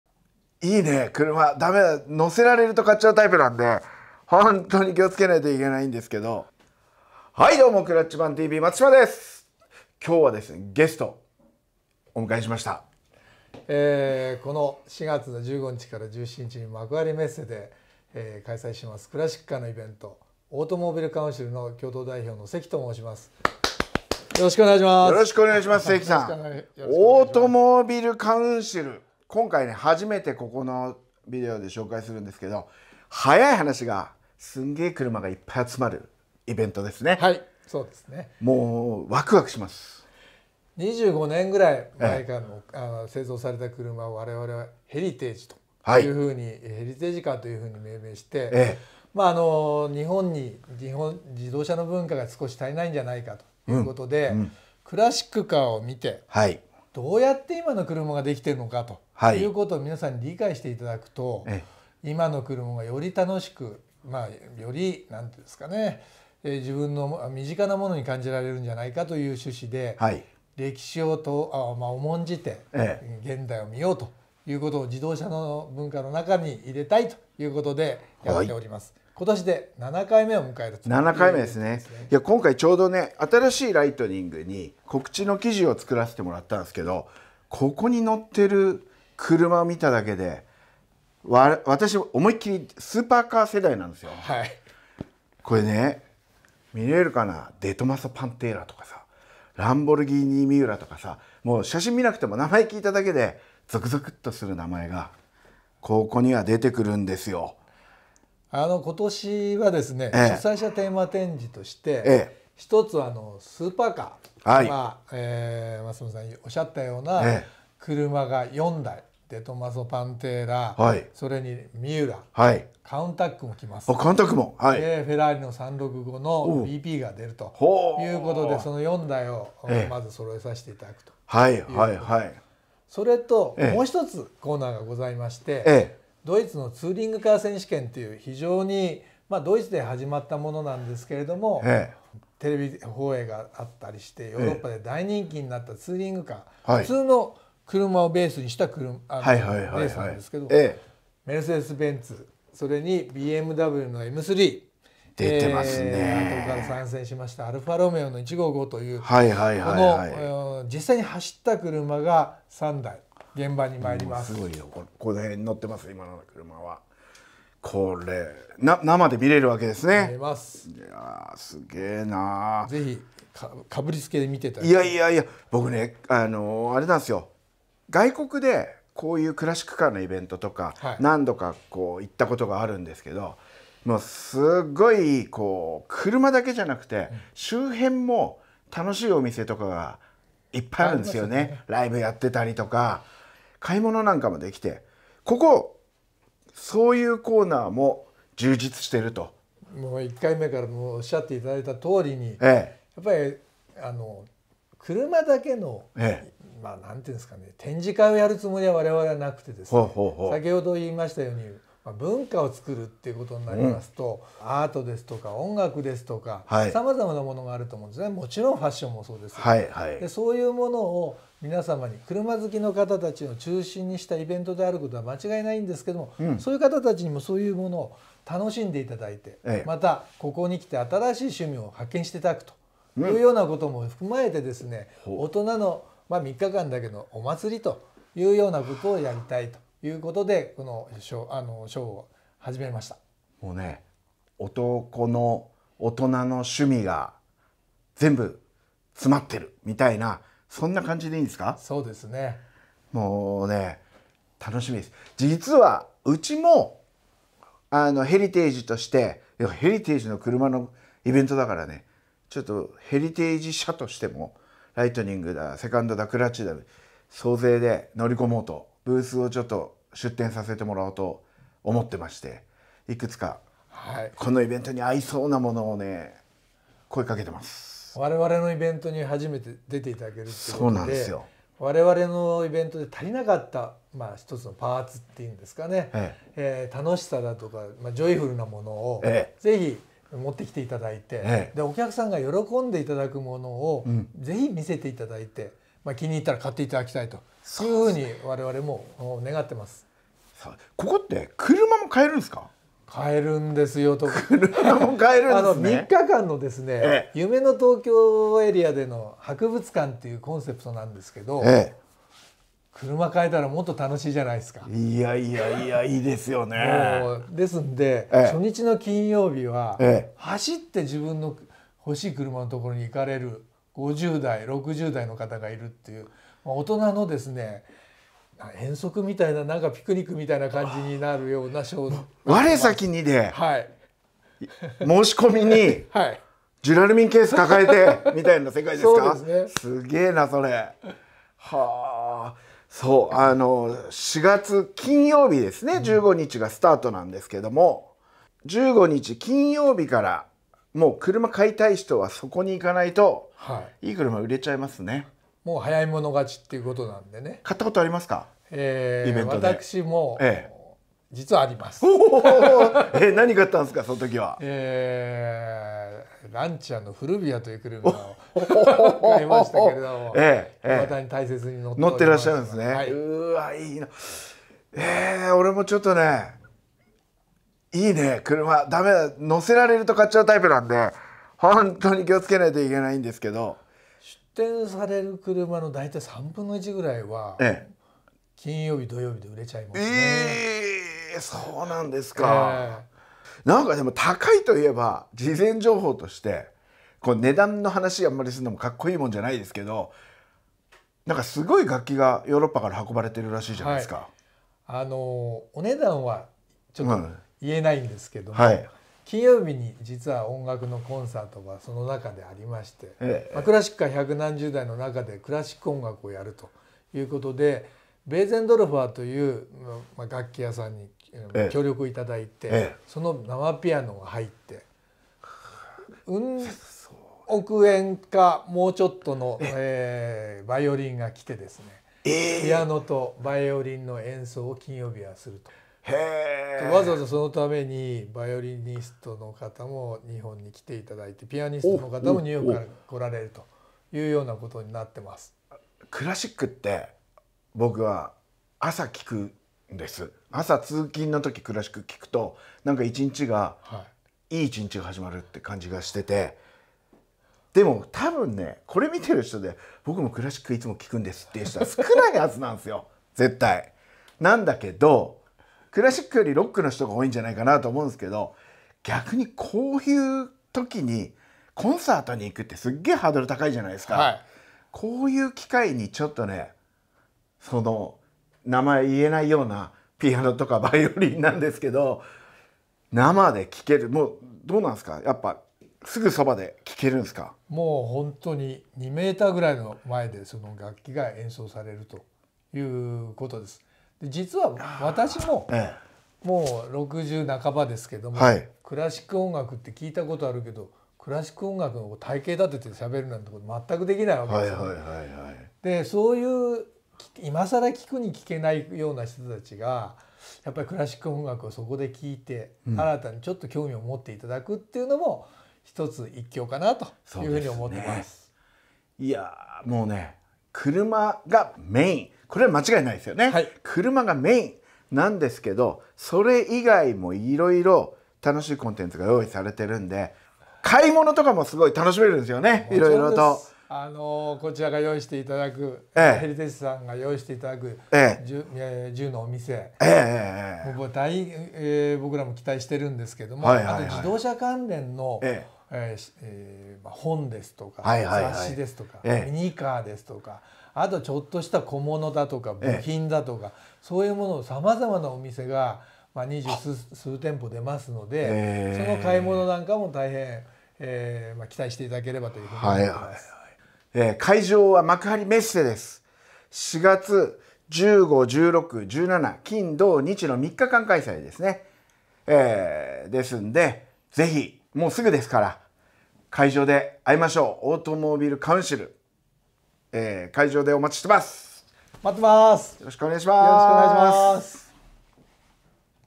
スーパーカー世代が大興奮！憧れの名車が集う！販売される！ビッグなイベントを開催。
AUTOMOBILE COUNCIL 2022（オートモビル カウンシル 2022）
テーマ展示：
DTM(ドイツ・ツーリングカー選手権)の主役たち
スーパーカードリーム
会 場：幕張メッセ (千葉県千葉市美浜区中瀬 2-1)
日 時：
4月15日（金）10：00～18：00（プレスタイム 10:00～12:30）
4月16日（土）10：00～18：00
4月17日（日）10：00～17：00
主 催：AUTOMOBILE COUNCIL 実行委員会